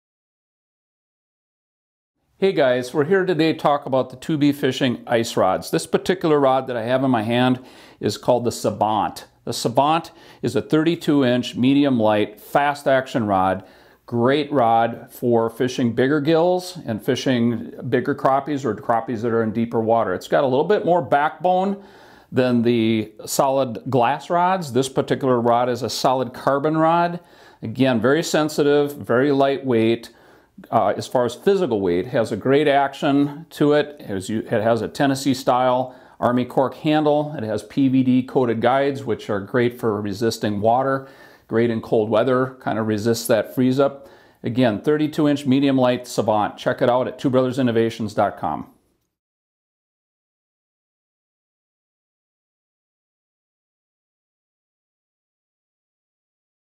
Savant - 2B Fishing
The Savant is a 32” solid graphite medium lite rod with a fast action that leans towards moderate. This rod is a workhorse and will become your “do everything” rod, perfect for working tiny rattle baits, small spoons and jigs & plastics.